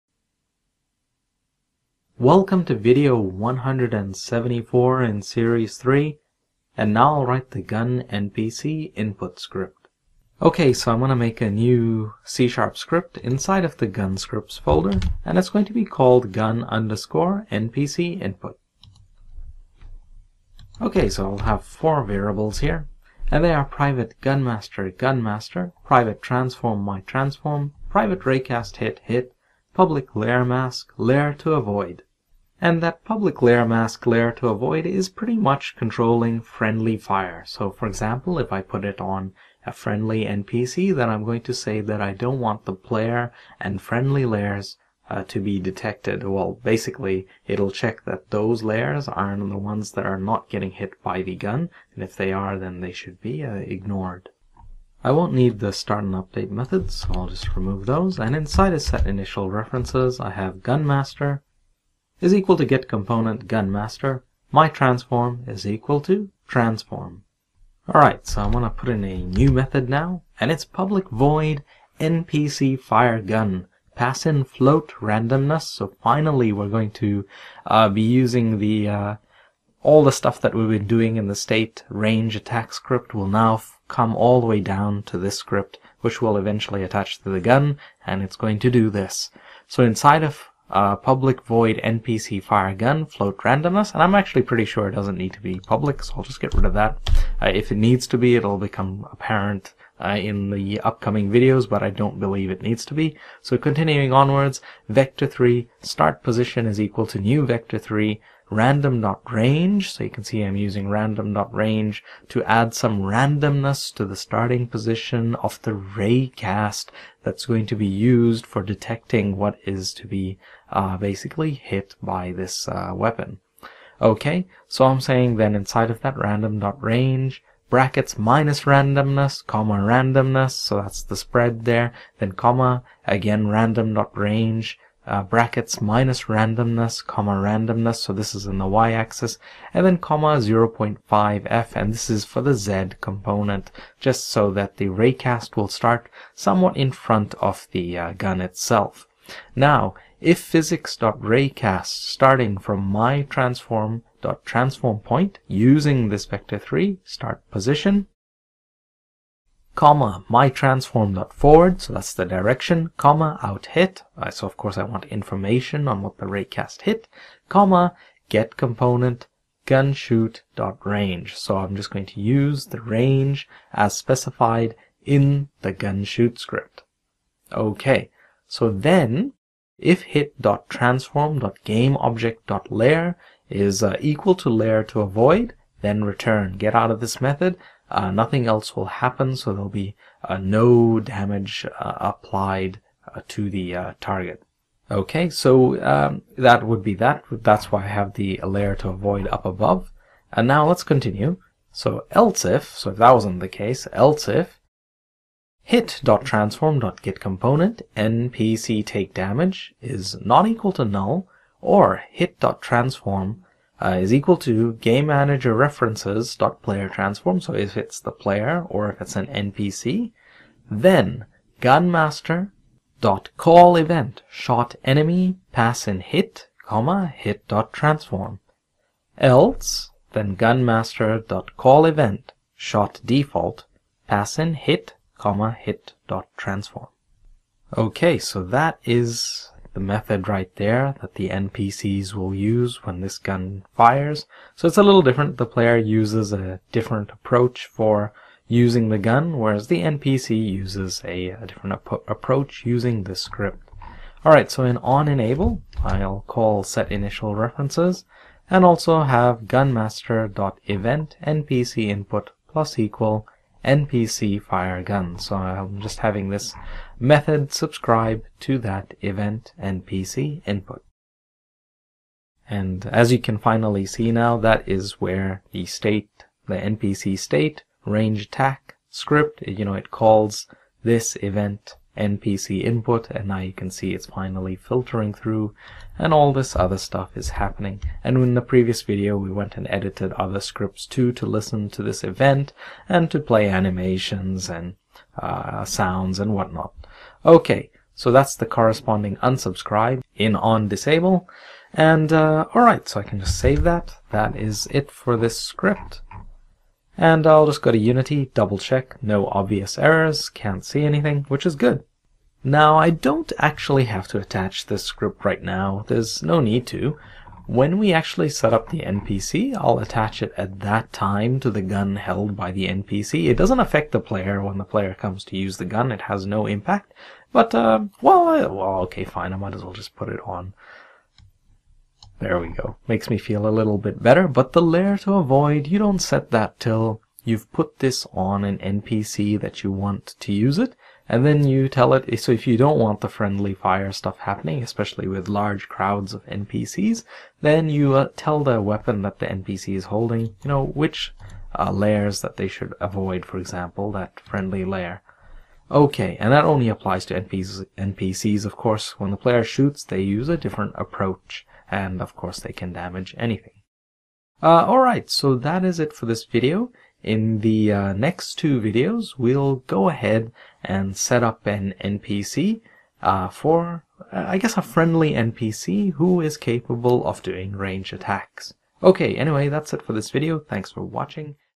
[174] Gun NPC Input - How To Make A Game
Now I write a script so that an NPC can use the gun.